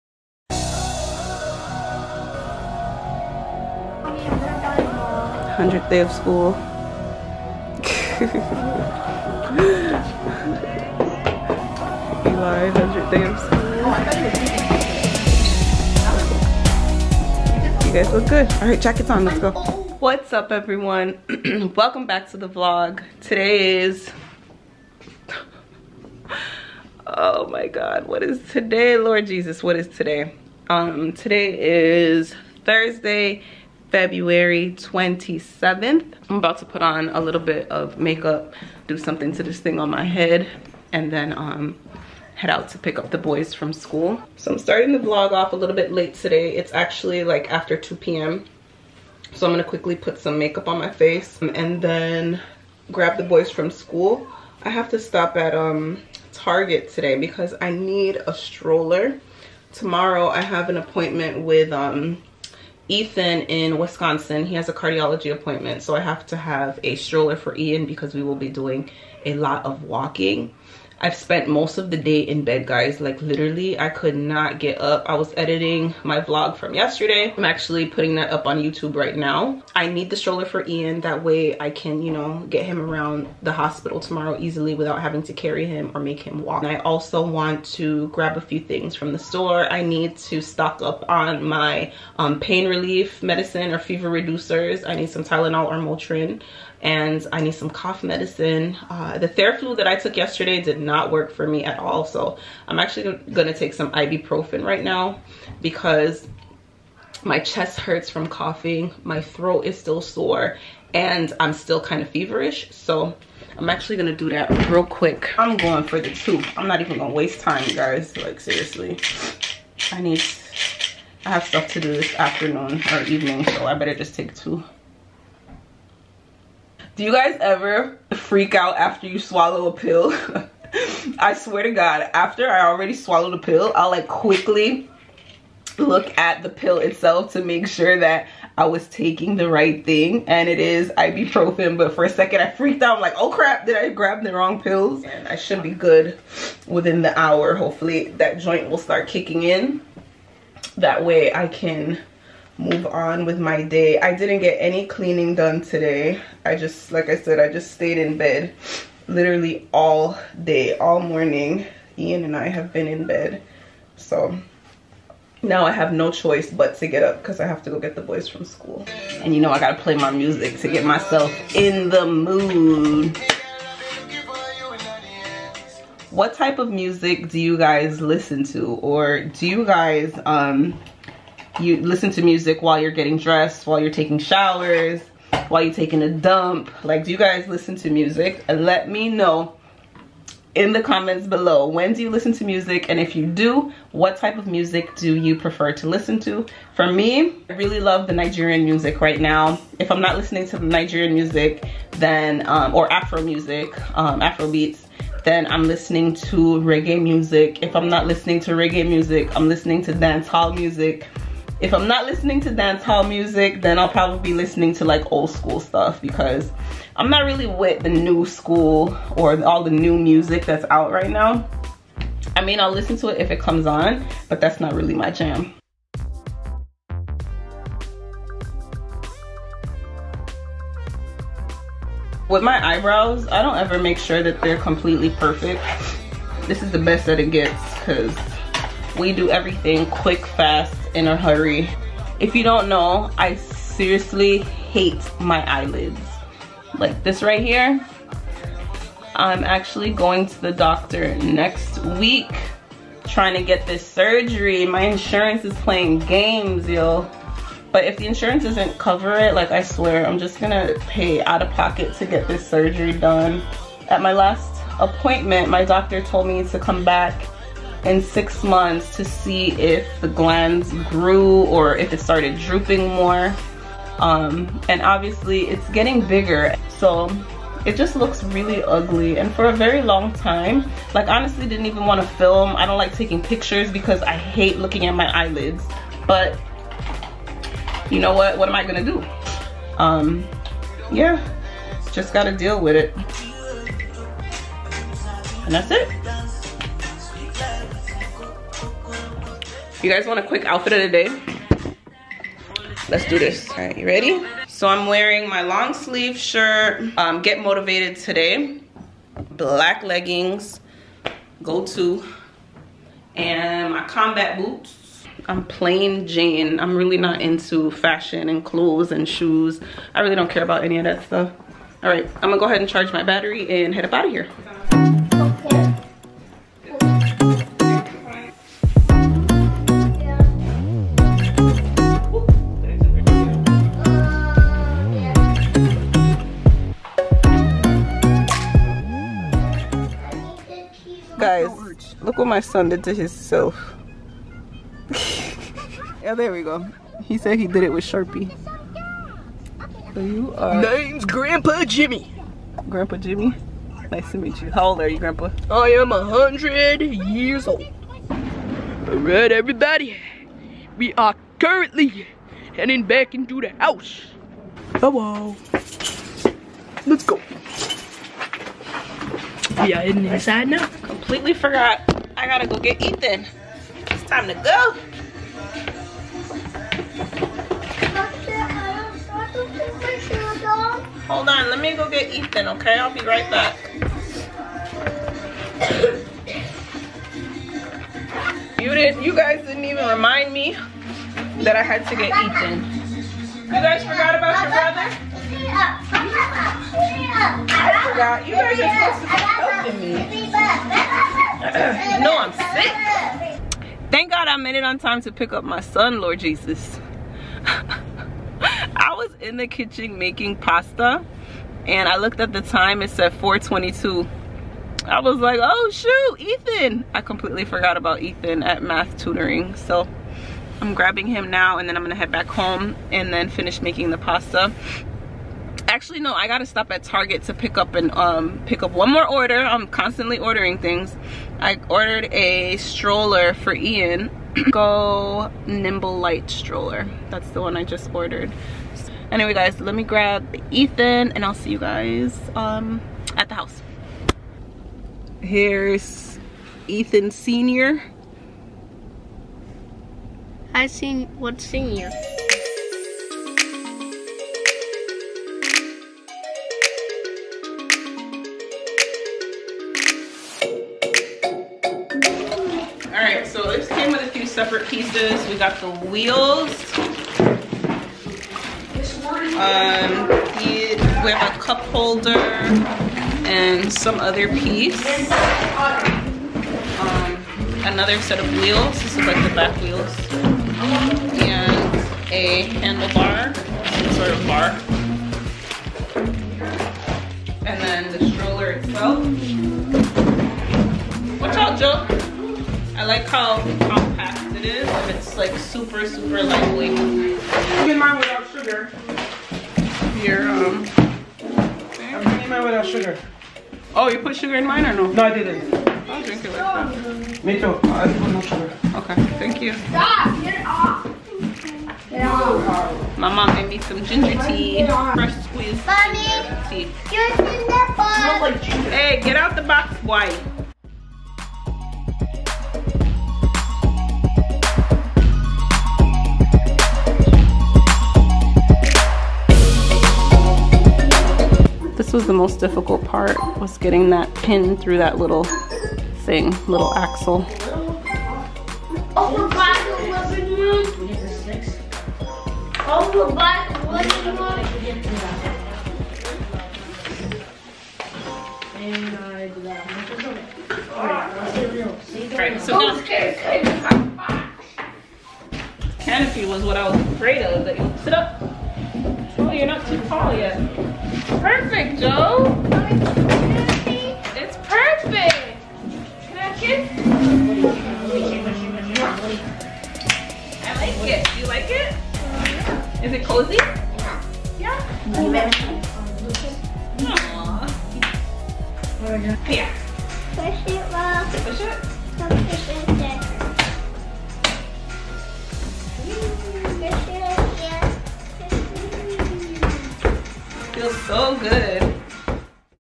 Another Sick Day and Eian's New Stroller
Check out Eian's new Graco NimbleLite stroller. It's very compact and maneuvers with ease. I'm in love!